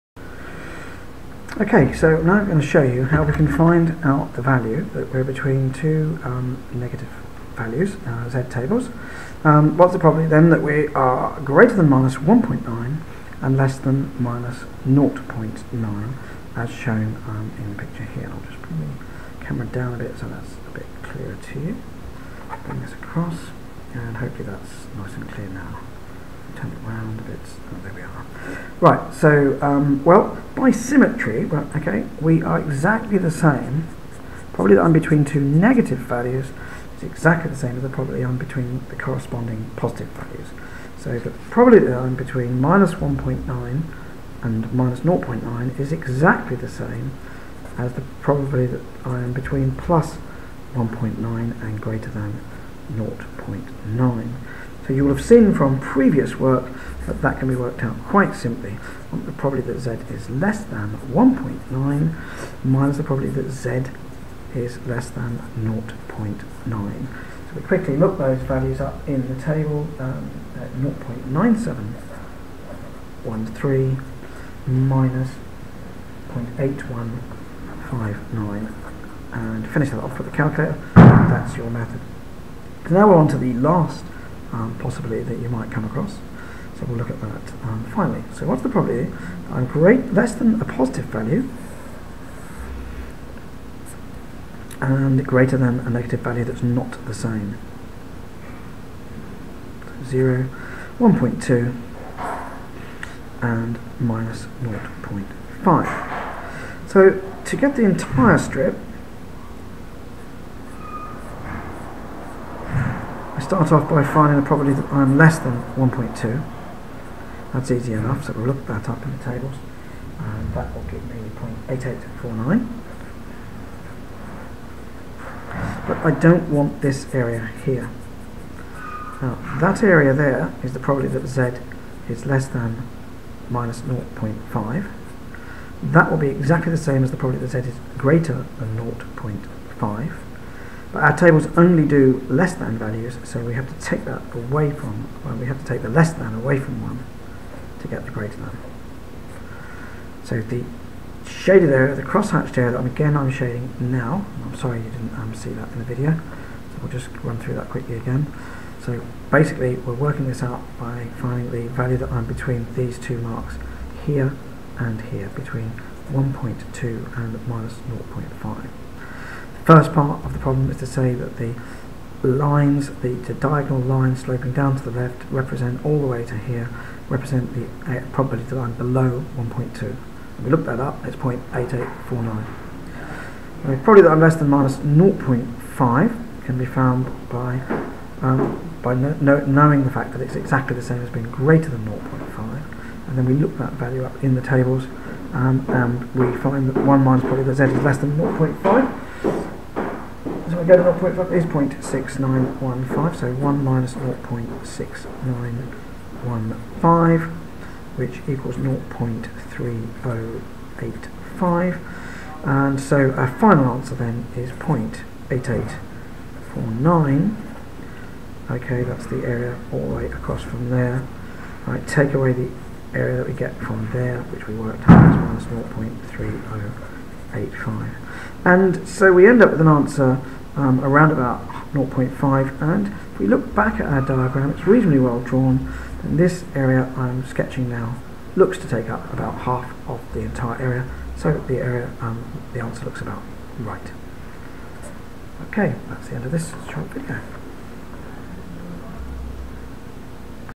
Normal Distribution Z tables
How to find out the probability that Z lies between a positive and a negative value